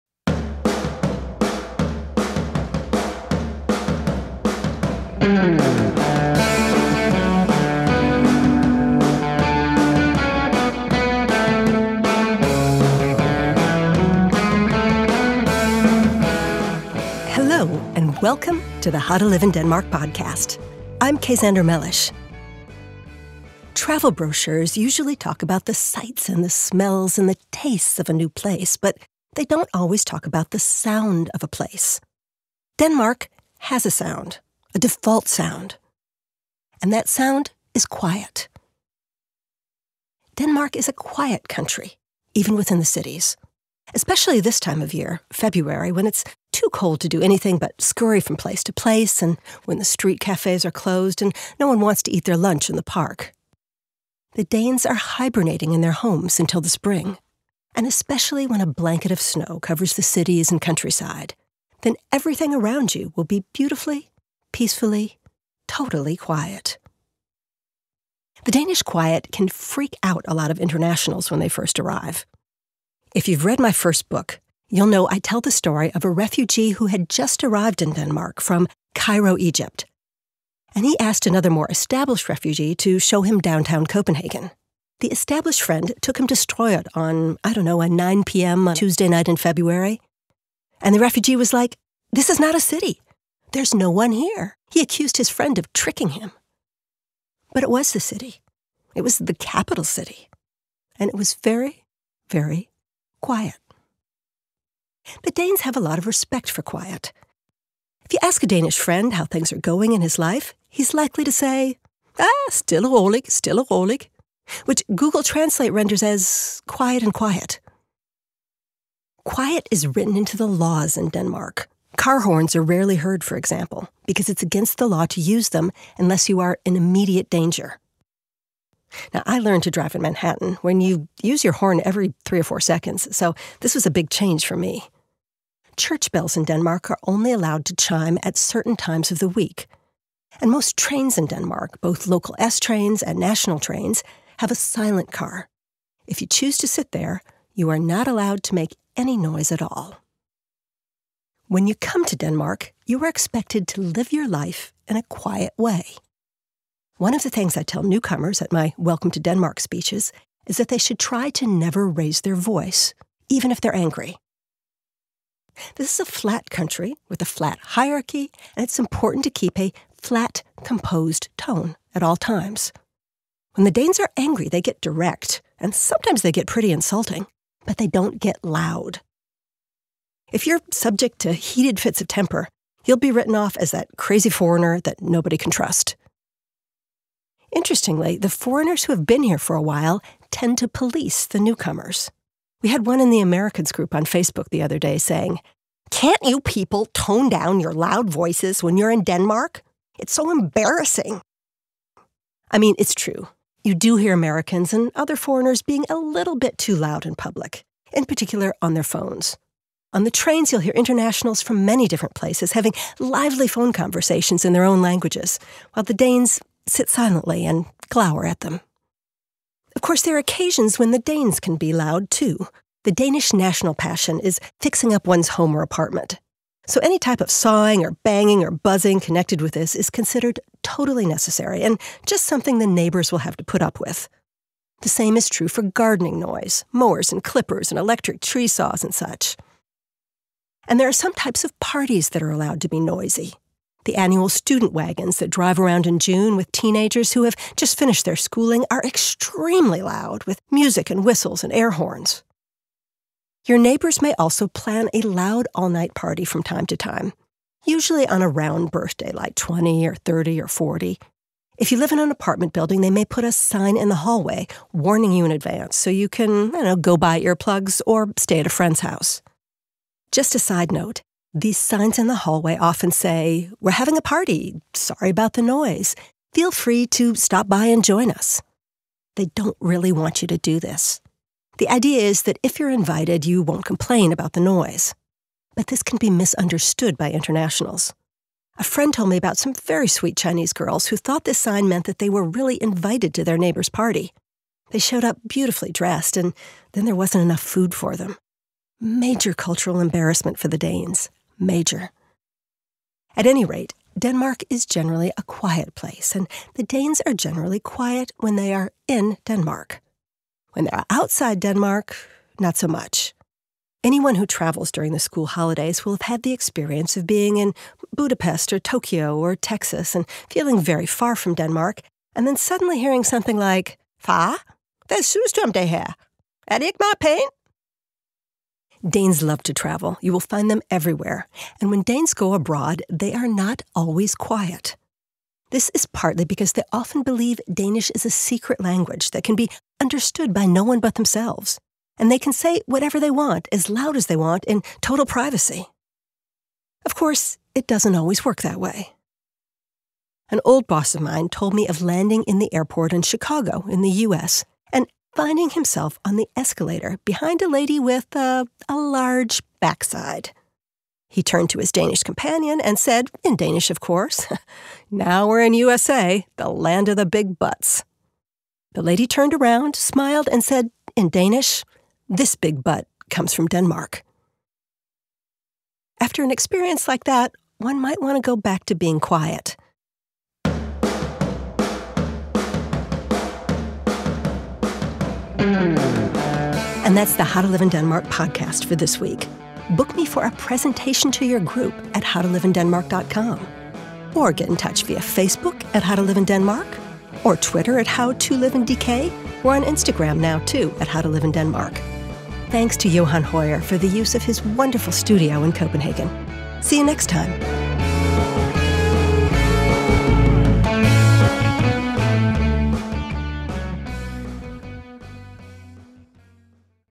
The sound of Denmark? Quiet. Very quiet | The How to Live in Denmark Podcast, Denmark's...
Denmark is a quiet country, even within the cities. Especially this time of year, February, when it’s too cold to do anything but scurry from place to place, when the street cafés are closed and no one wants to eat their lunch in the park. The Danes are hibernating in their homes until the spring.

And especially when a blanket of snow covers the cities and countryside. Then everything around you will be beautifully, peacefully, totally quiet.

This Danish quiet can freak out a lot of internationals when they first arrive.

The Danes have a lot of respect for quiet. If you ask a Danish friend how things are going in his life, he’s likely to stay “Ah, stille og roligt.” Which Google Translate renders as “Quiet and quiet.” (‘Nice and easy’ is another translation)

Quiet is written into the laws in Denmark – car horns are rarely heard, for example, because it’s against the law to use them unless you are in immediate danger. I learned to drive in Manhattan, where you use your horn every 3 or 4 seconds, so this was a big change for me.

Church bells are only allowed to chime at certain times of the week. And most trains in Denmark – local S-trains and national – have a silent car. If you choose to sit there, you are not allowed to make any noise at all.

This is the eighty-fourth episode of the "How to Live in Denmark podcast", and originally ran in 2019.

Get all of Kay Xander Mellish's books on Denmark Book Kay for a talk to your group or organization